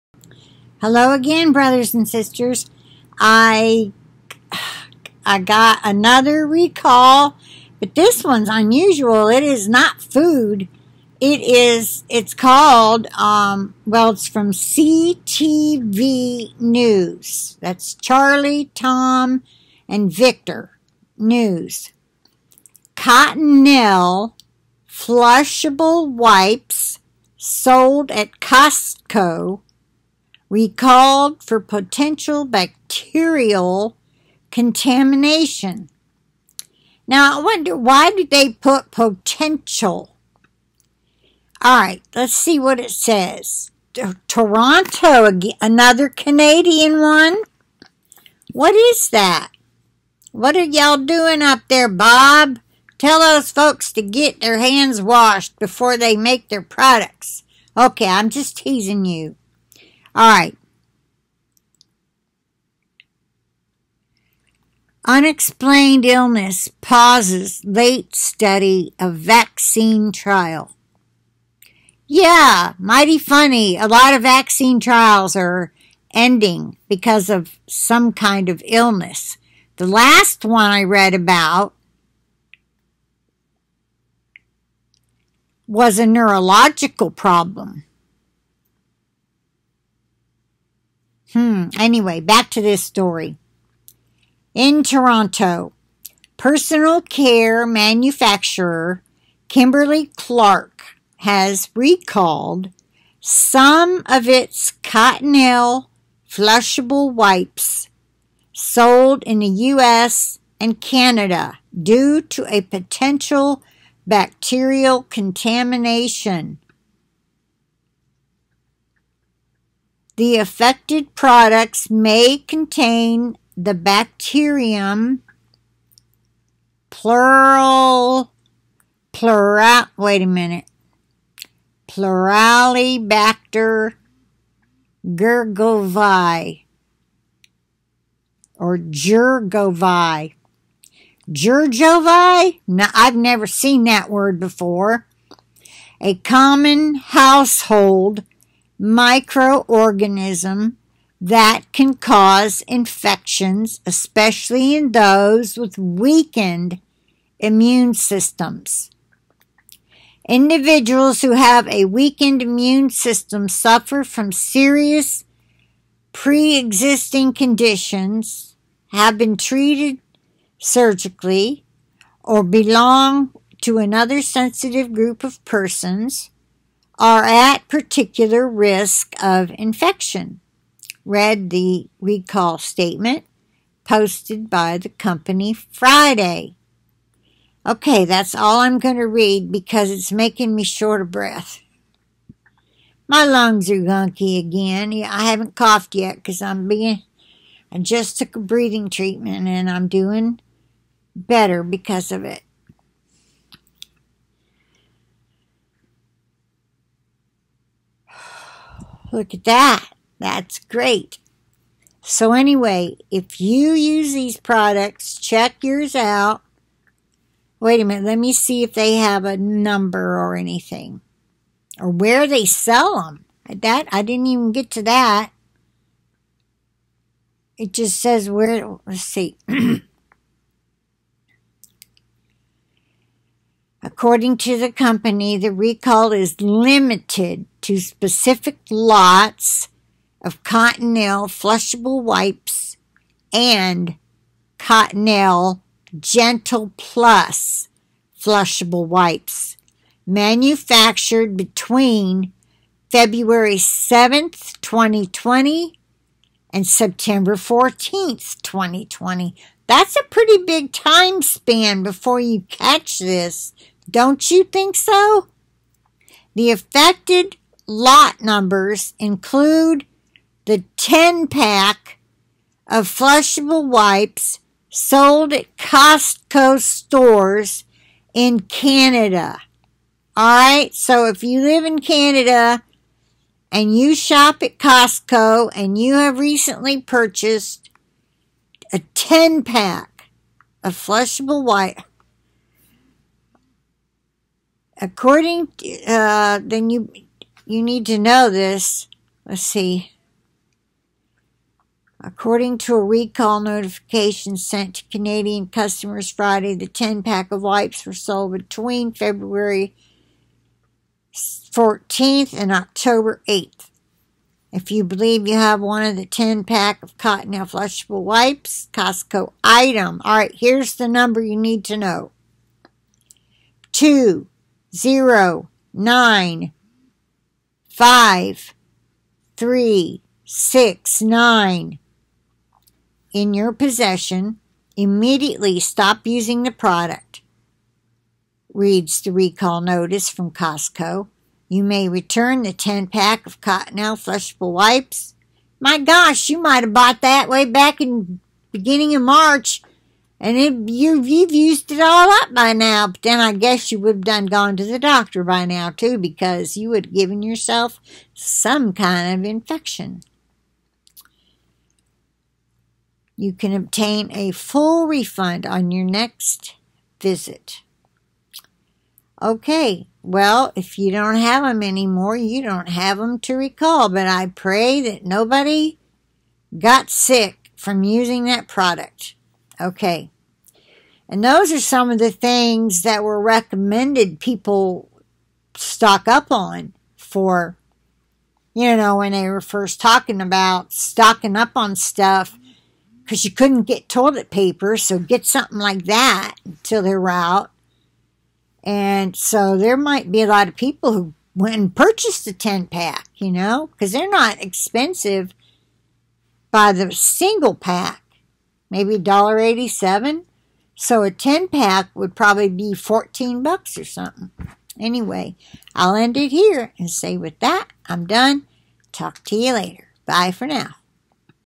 COTTONELLE WIPES SOLD IN 10-PACKS, IN CANADA, BEING RECALLED FOR POTENTIAL BACTERIAL CONTAMINATION
Title: "Cottonelle flushable wipes sold at Costco recalled for potential bacterial contamination"

“If you believe you have one the 10 pack of Cottonelle Flushable Wipes (Costco Item 2095369) in your possession, immediately stop using the product,” reads the recall notice from Costco.